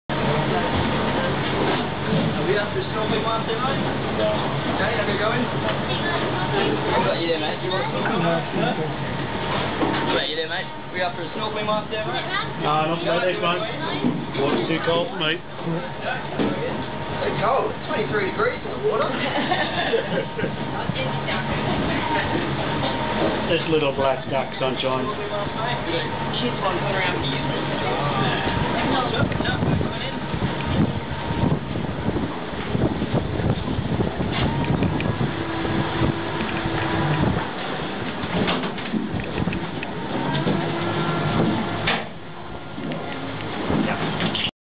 ningaloo reef glass bottom boat coral view pt1
Glass bottom boat  coral reef viewing on the Ningaloo Reef